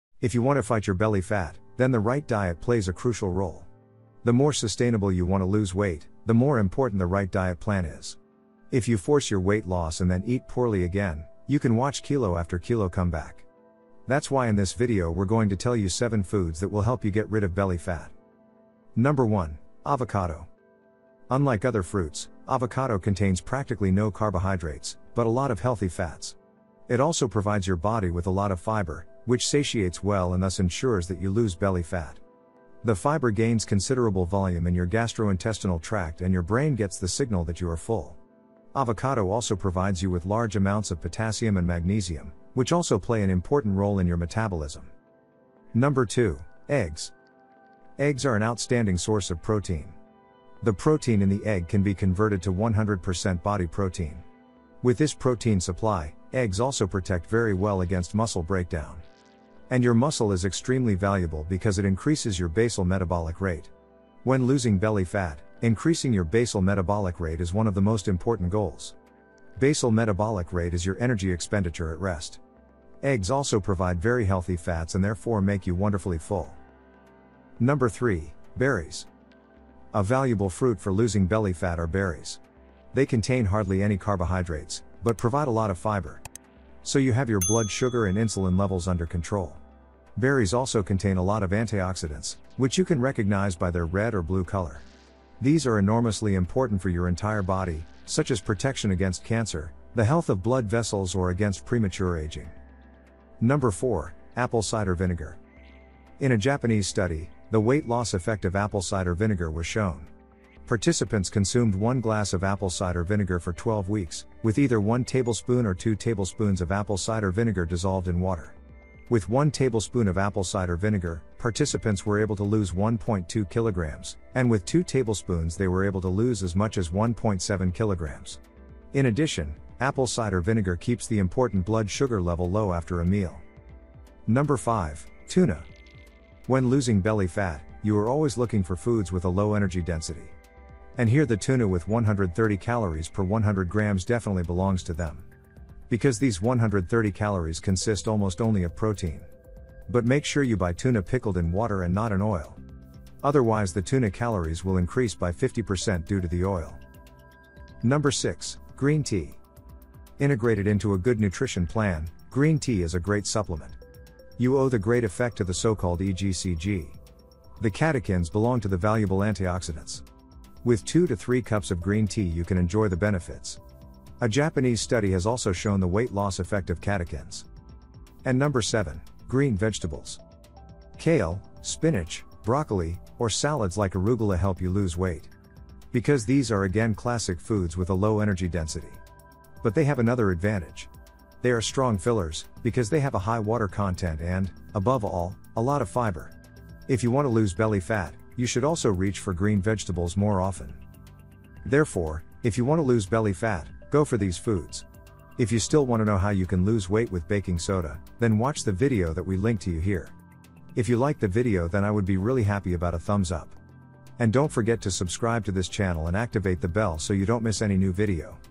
How to lose Belly Fat with THESE 7 Foods 💥 (INSANE Easy) 🤯
Do you also want to lose some belly fat? - Here we show you which foods you need to eat to lose belly fat!

Is your biggest problem area the belly and you want to lose some belly fat? Then it is important that you eat the right foods. Because there are also foods that help you lose belly fat. Therefore, in this video we will show you 7 foods that will help you lose belly fat faster and easier. Watch the video and eat these foods to lose belly fat!